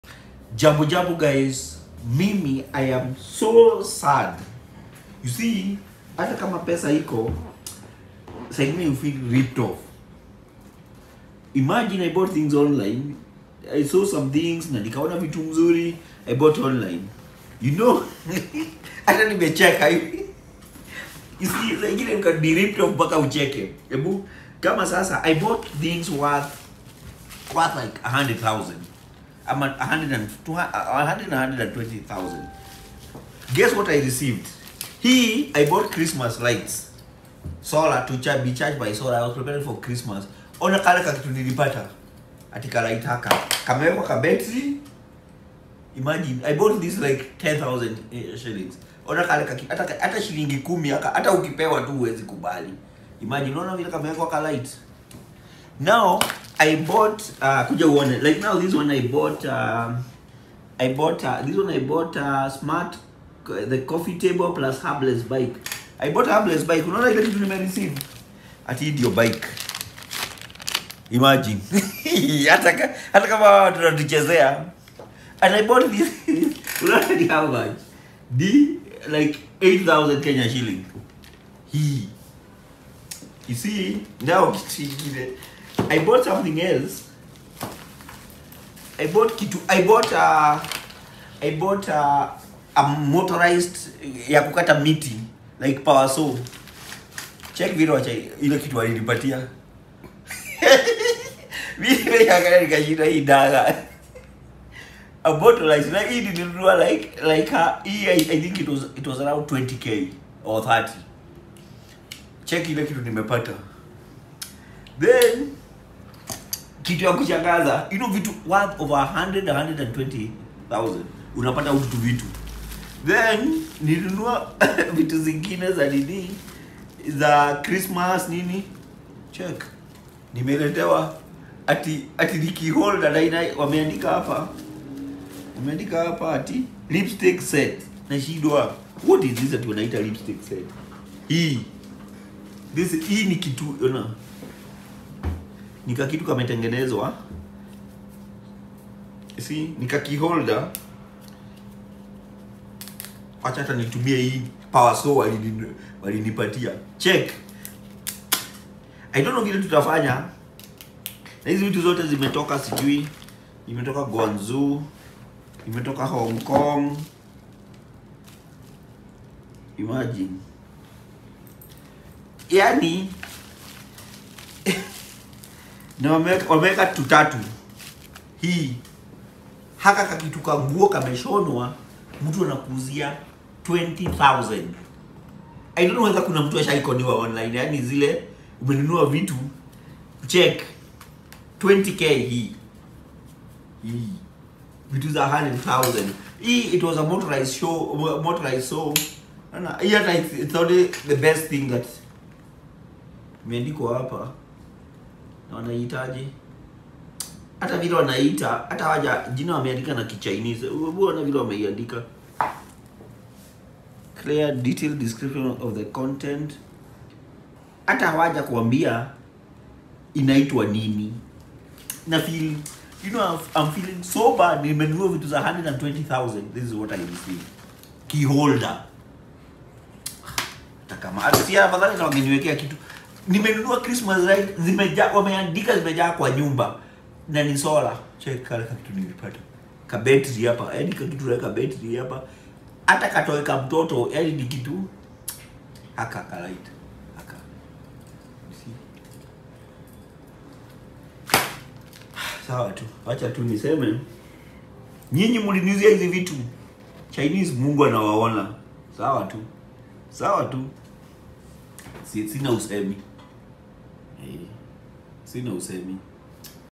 The way i was conned online /Simon Kabu.
I paid over one hundred thousand online only to receive a key holder.
Have you ever been conned and how did it go?